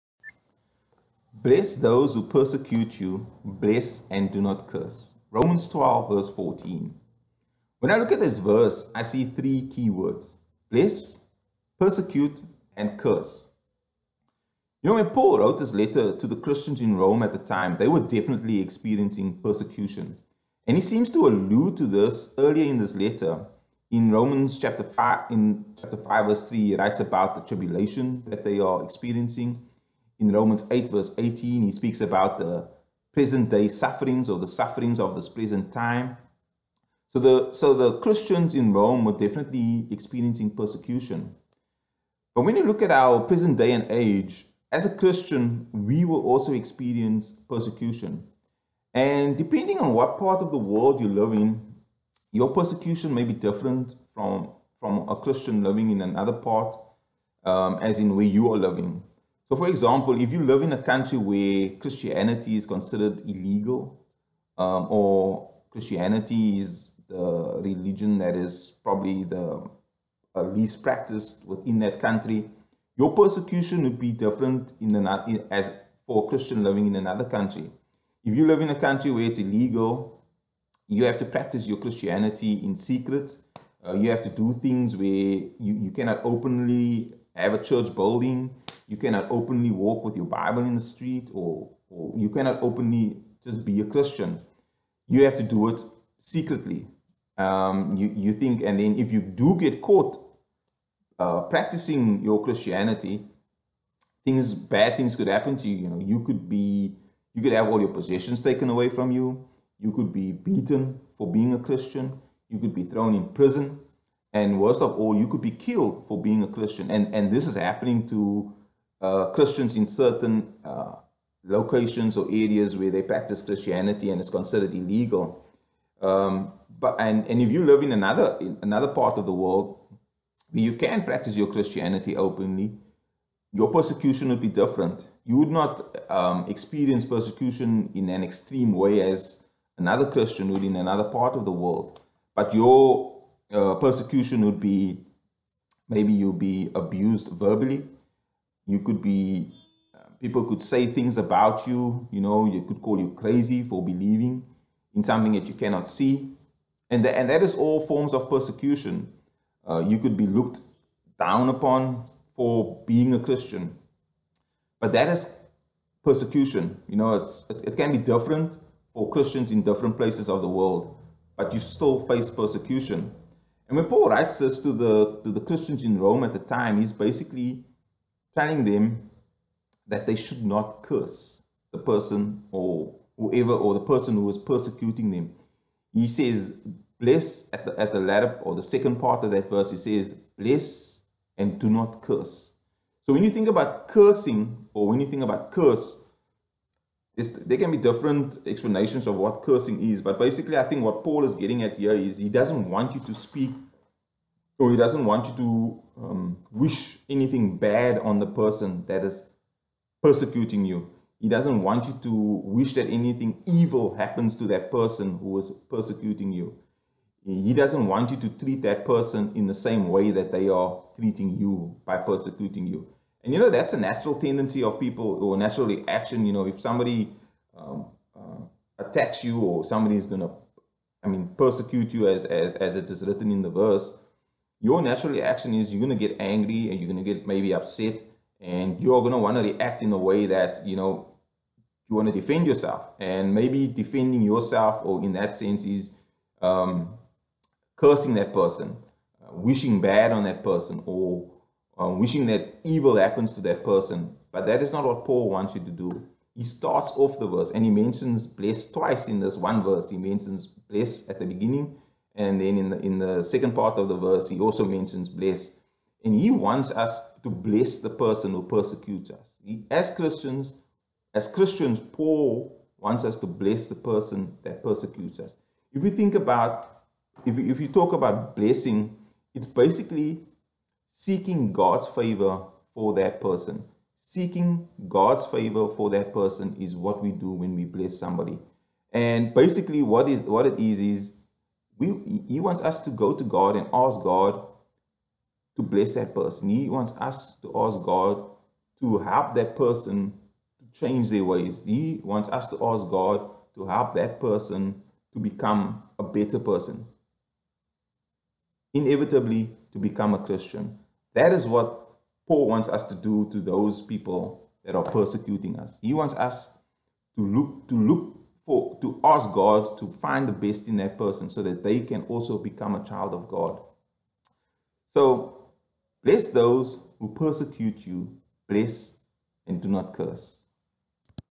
Bless Those Who Persecute You
Josiah Carelse brings this devotional thought to us from Romans 12:14. "Bless those who persecute you; bless and do not curse them."